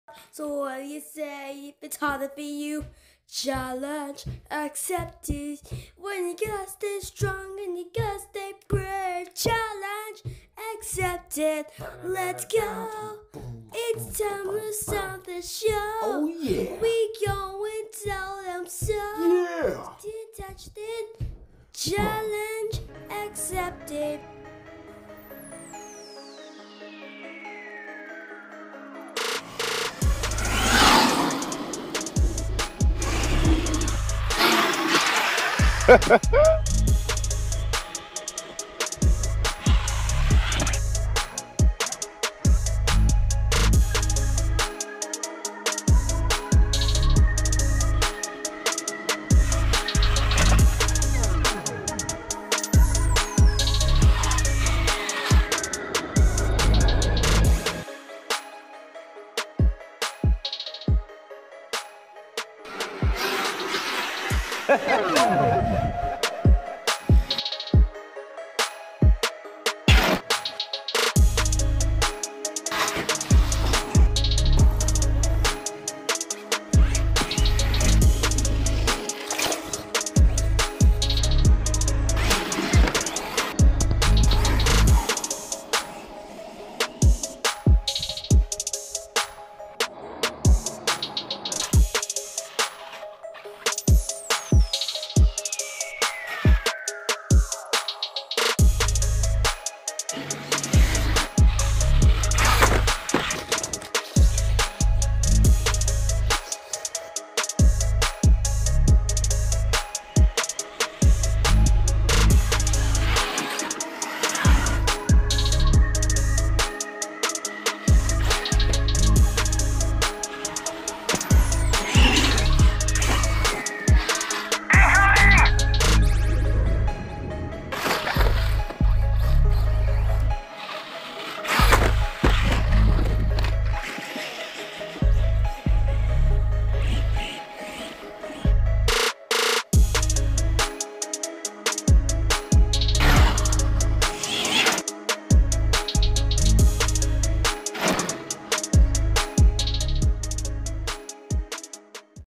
NEW RC BASH! Losi Super Baja Rey on paddles & Arrma Mojave bashing hard at DJ's Place Motocross.
Lost file of two high powered RC's going at it at DJs Place Motocross in Wrens, GA. Filmed with DJi Mavic Zoom drone! Thanks to Rev Kev & Leonidas the Bold, two of Gladiator RC finest drivers!